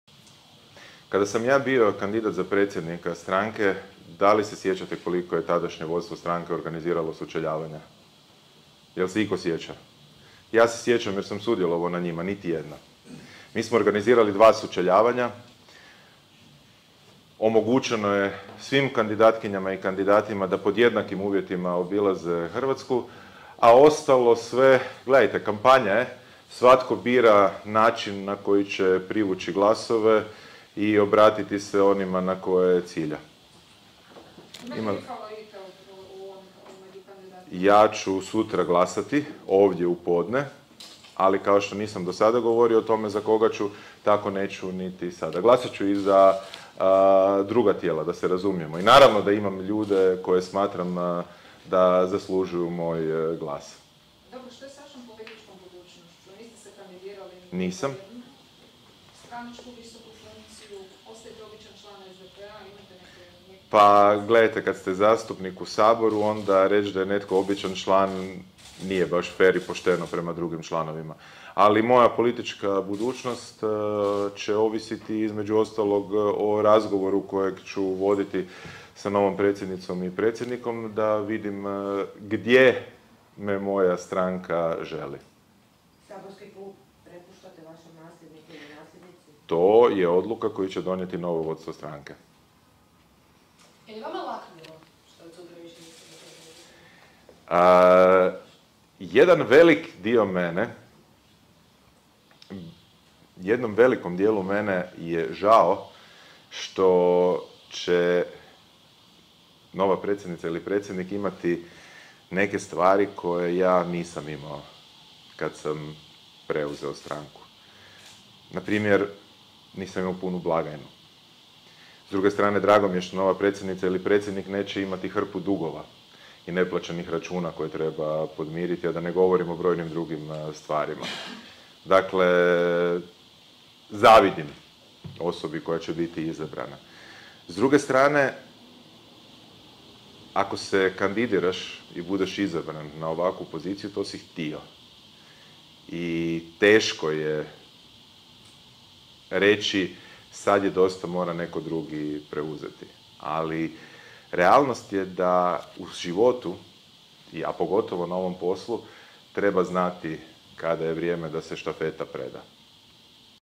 Grbin: "Moja politička budućnost će ovisiti o novom predsjedniku ili predsjednici SDP-a"
U središnjici SDP-a na Iblerovom trgu, predsjednik stranke Peđa Grbin, uoči sutrašnjih stranačkih izbora održao konferenciju za medije.